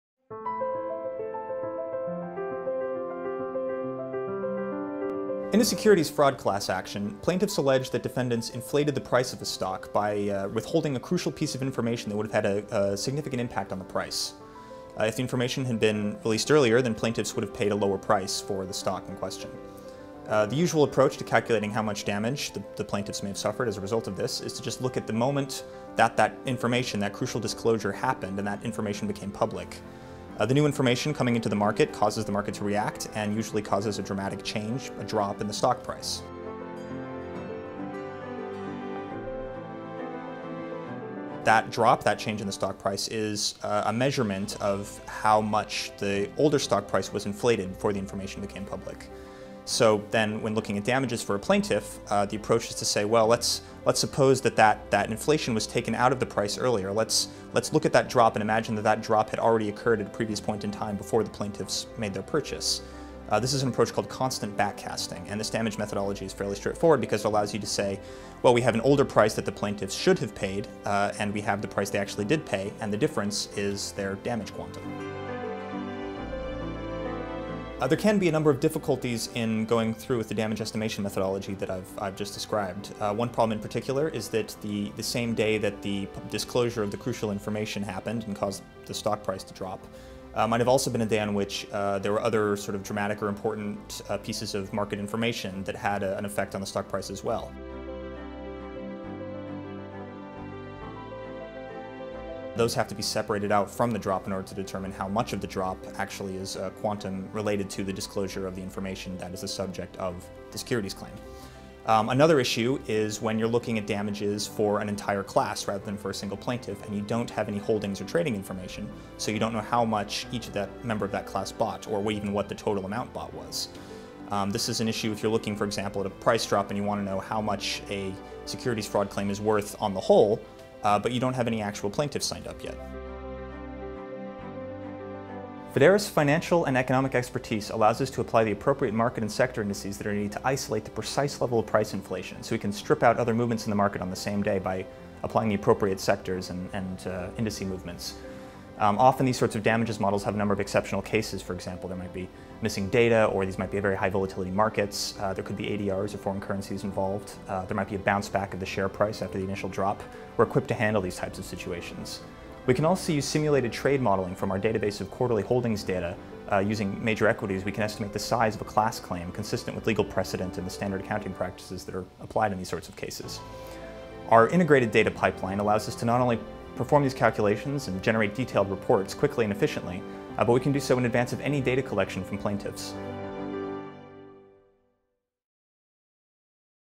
Event Studies in Securities Fraud Claims
In this webinar, Fideres's Director Max Theiler discusses how Event Studies are used for securities fraud damages calculations and issues that may arise in the use of this methodology. For more similar content, visit our